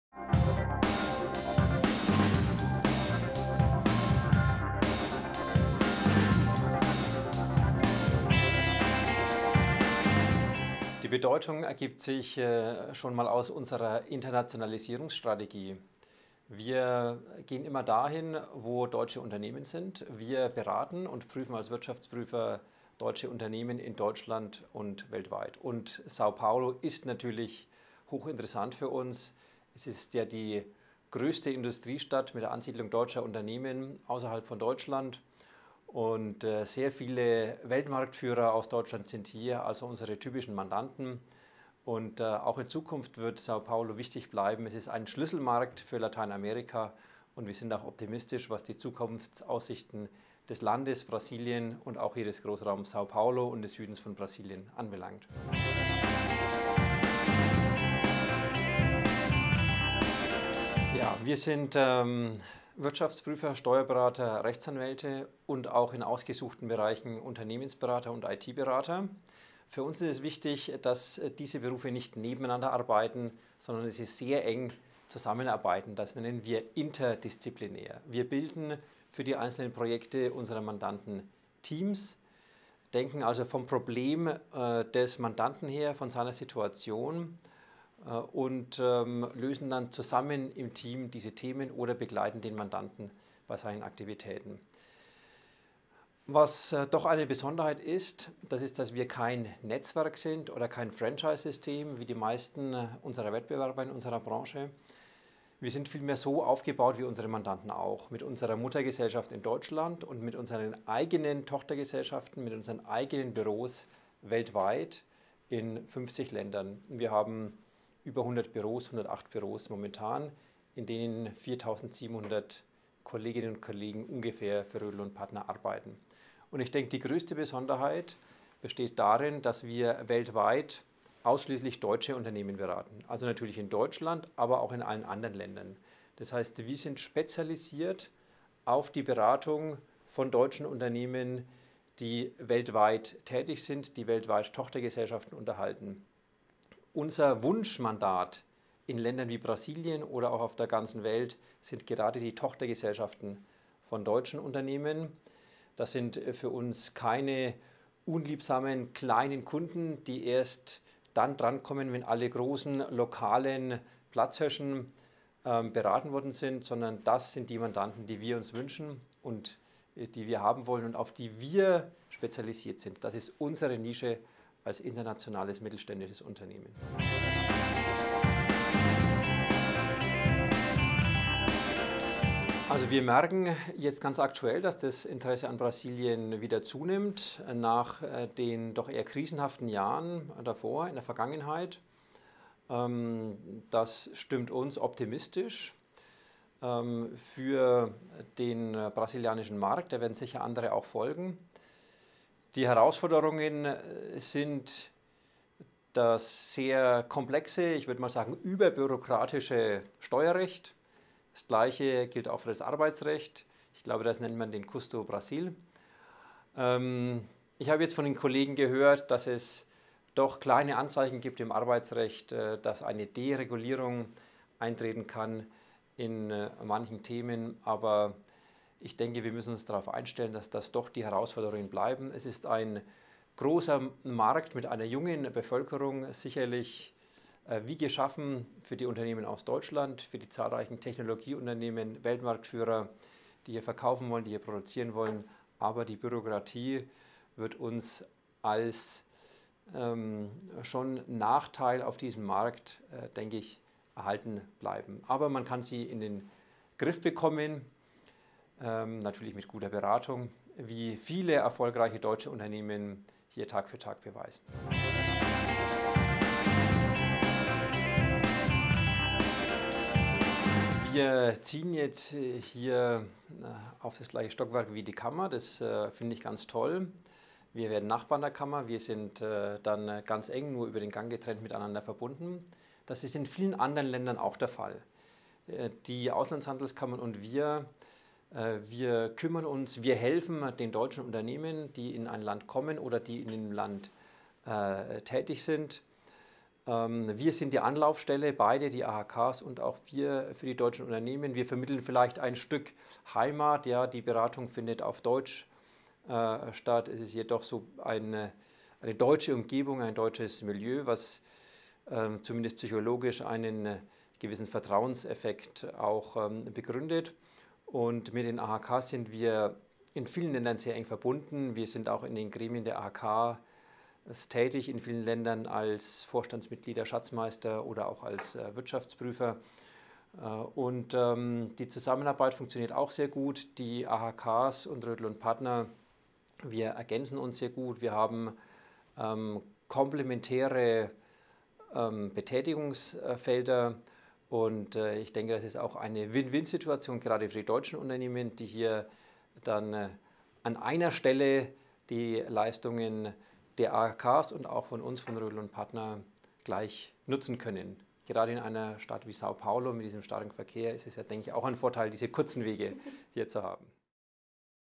Entrevista com Prof. Dr. Christian Rödl - Associada Rödl & Partner
Dr. Christian Rödl, Sócio Gerente e Presidente do Conselho de Administração da nossa associada Rödl & Partner, nos concedeu uma entrevista apontando a importância do mercado brasileiro, as expectativas da empresa no Brasil, além da cooperação entre a câmara e a Rödl & Partner.
Confira a entrevista completa!